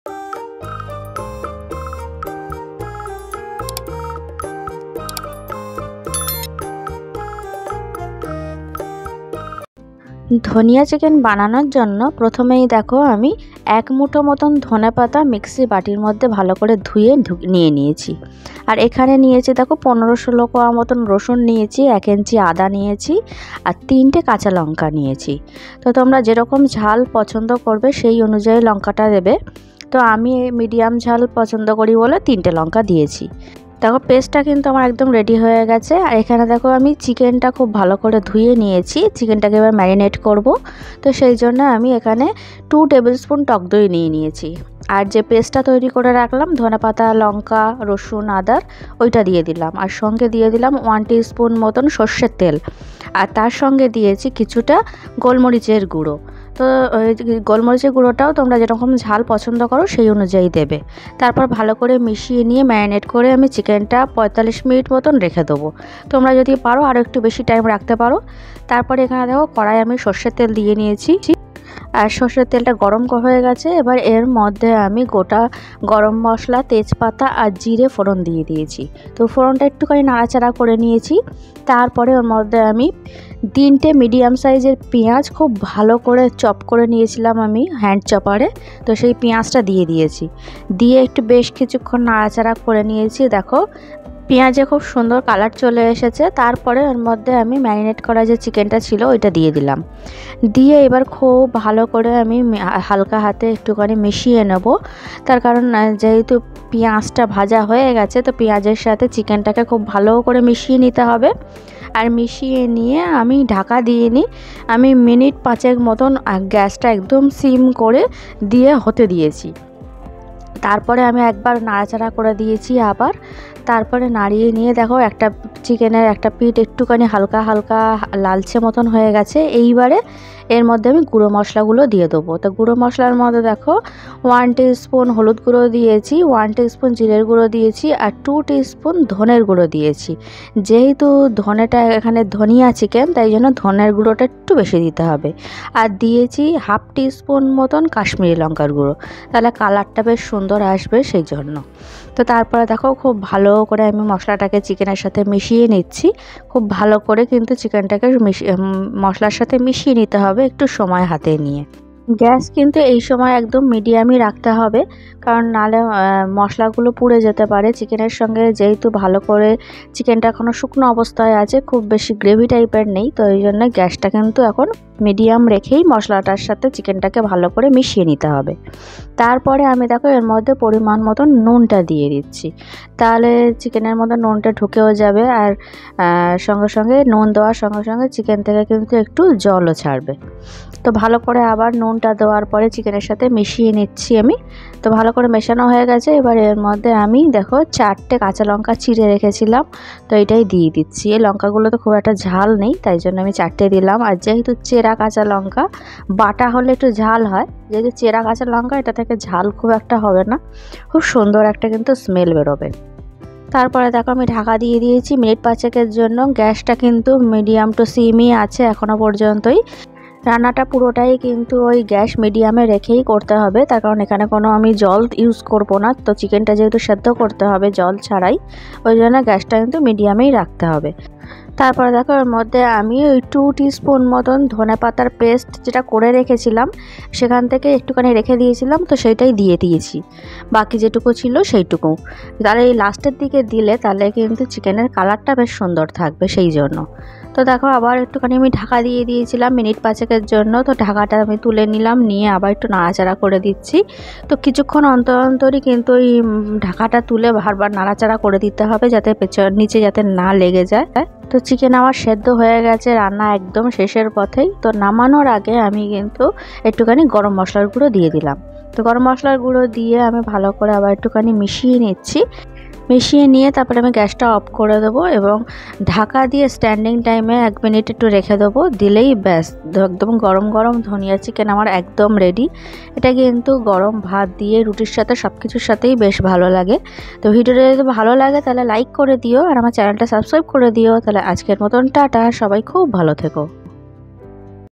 শীতকাল স্পেশাল ধনিয়া চিকেন রেসিপি।।Winter special dhonia chicken recipe||Coriander chicken curry
bengalivlog

How spend a day with my baby girl. My cooking.